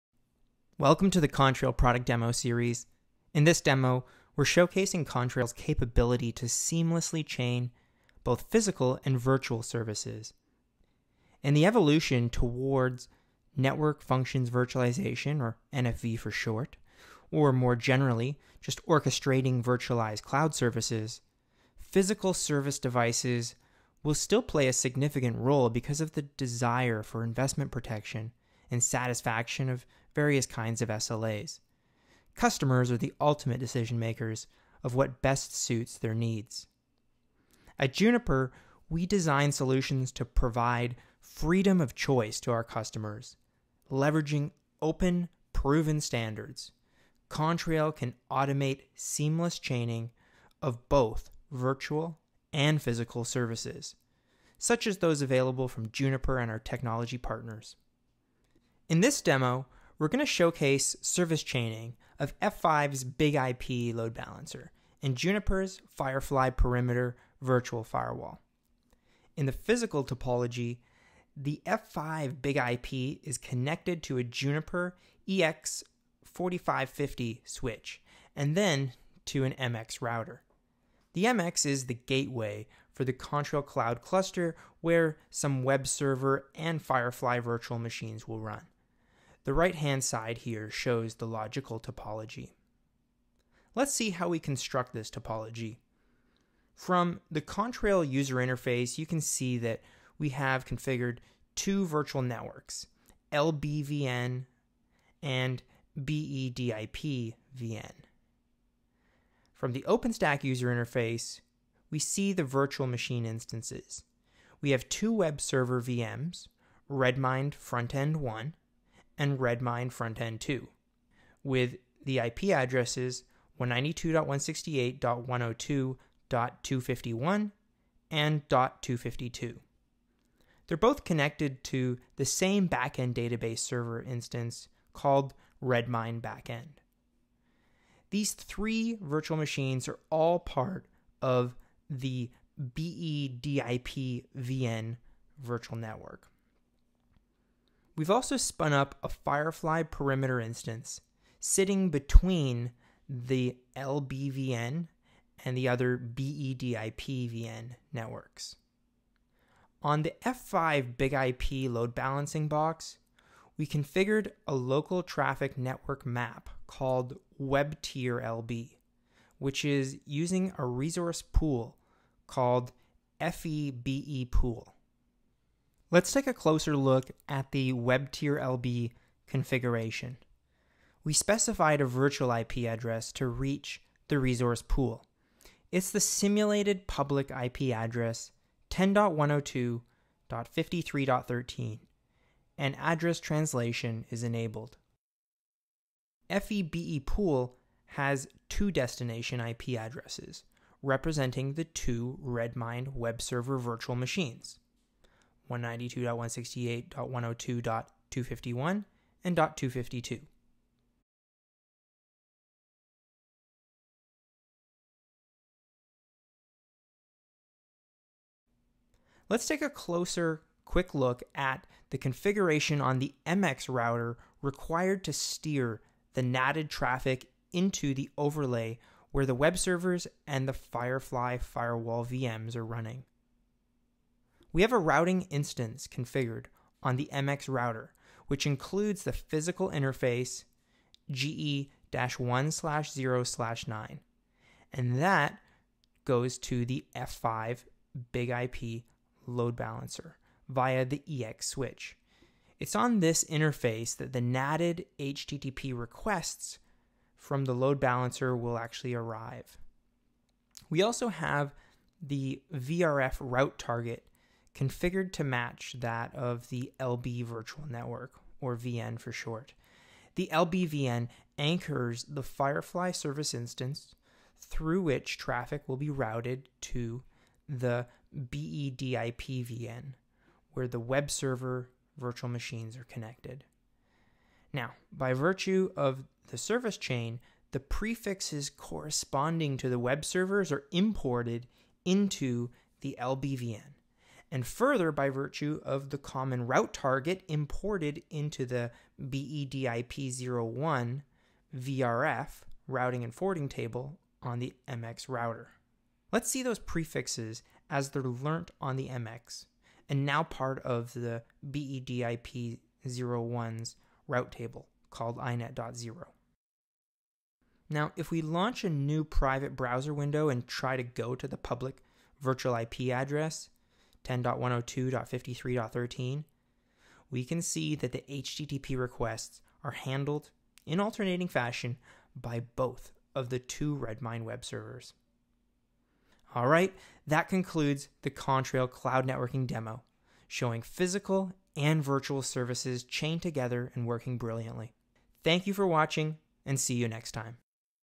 Seamless Chaining of Physical and Virtual Services with F5 BIG-IP and Juniper Firefly by Contrail
This video showcases Contrail’s capability to seamless chain both physical and virtual services from Juniper and our technology partners to provide freedom of choice to our customers.